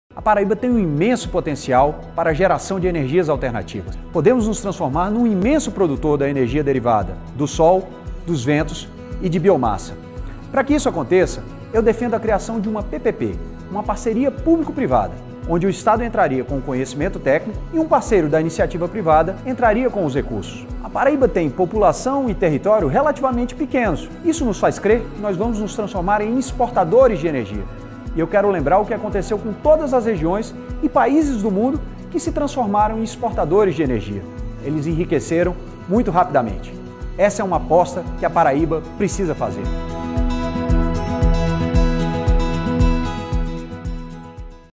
Leonardo Gadelha defende PPV para o estado da Paraíba
Leonardo Gadelha (PSC-PB) defende uma Parceria Público Privada (PPV) para utilizarmos energias alternativas no estado da Paraíba.